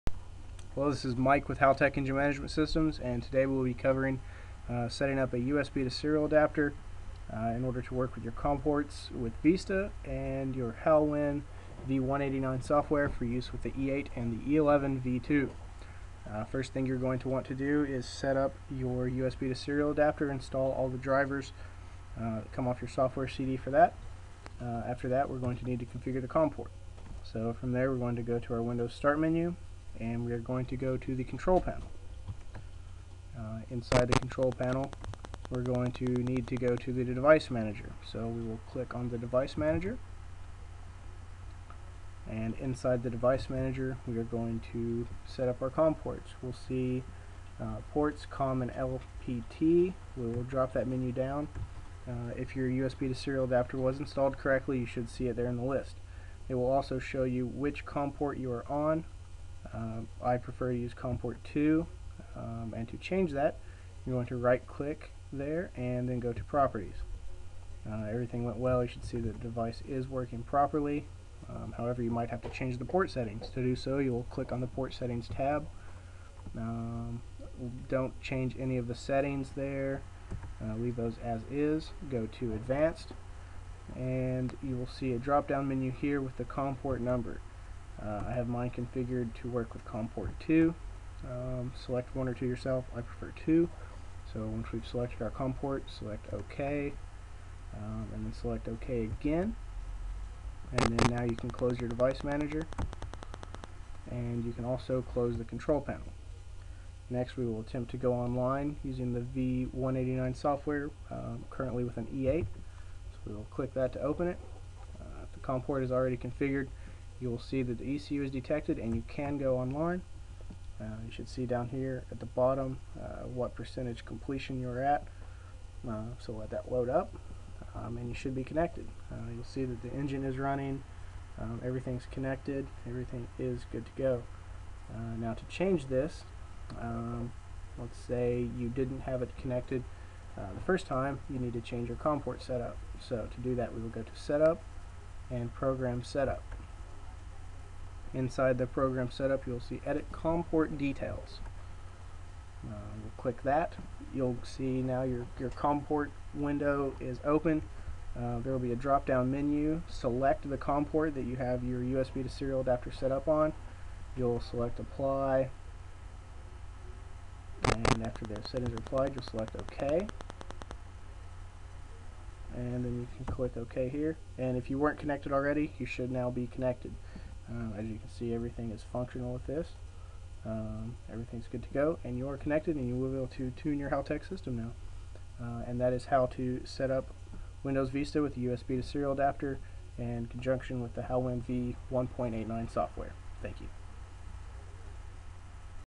USB to Serial Adapter with Haltech and VIsta 1.89 Software
This tutorial shows you how to setup a USB to serial adapter in Windows Vista for use with the Halwin 1.89 Software.  This is for use with the E8 and E11v2.